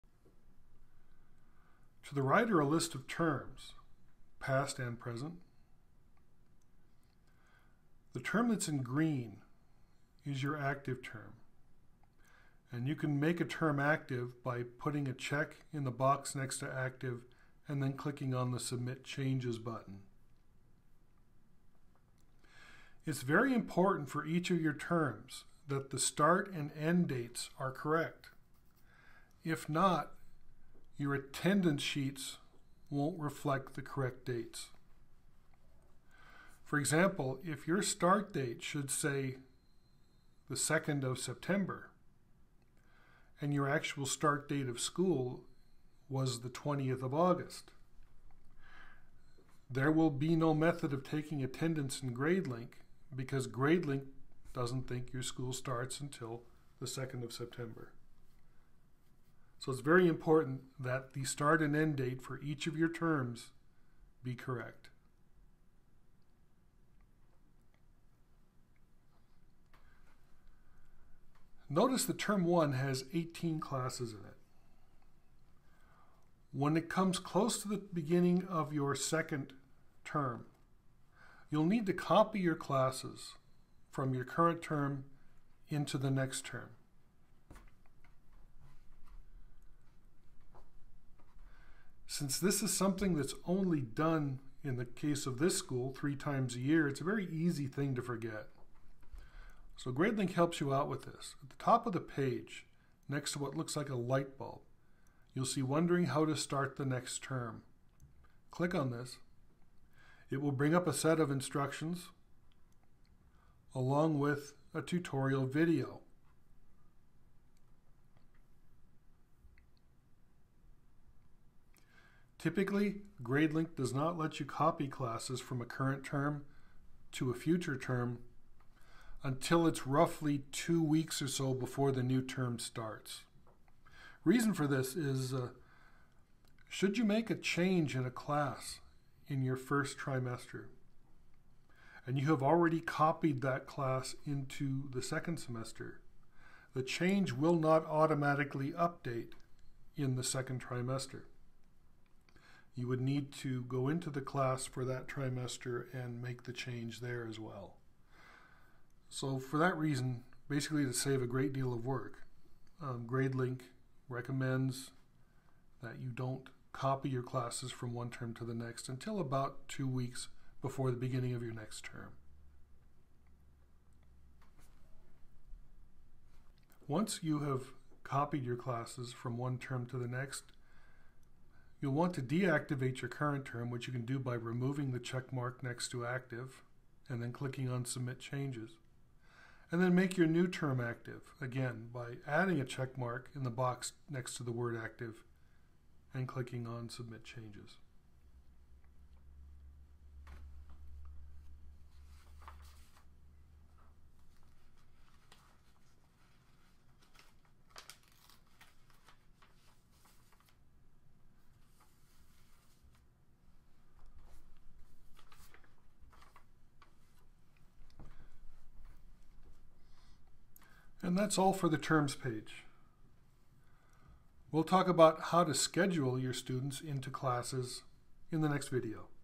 How to create a new Term (Quarter, Trimester) or a new school year.
This video is a quick tutorial on how to create new terms, trimesters, or even a brand new school year in Gradelink. It's easy, user-friendly, and best of all - fast!